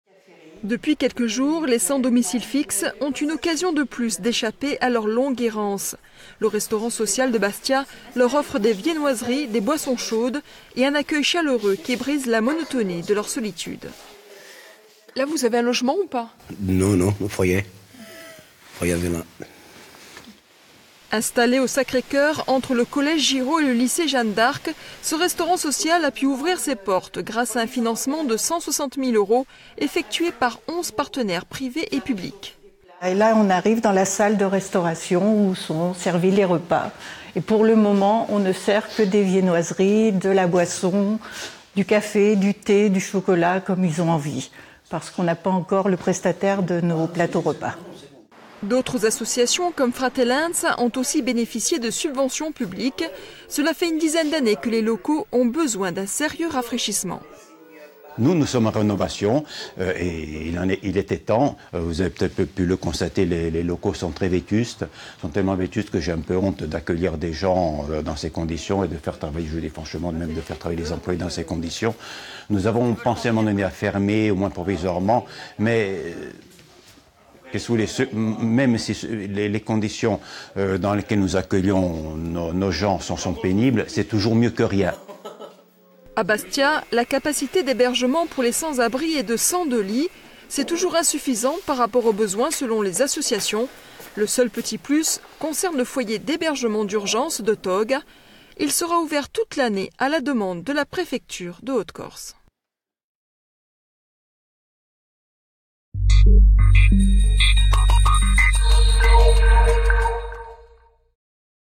Bastia : ouverture d'un restaurant social pour les plus démunis
A Bastia, les structures d'accueil pour les plus démunis se renforcent. Un restaurant social a ouvert, accessible aux sans domicile fixe mais aussi aux personnes à faible revenu.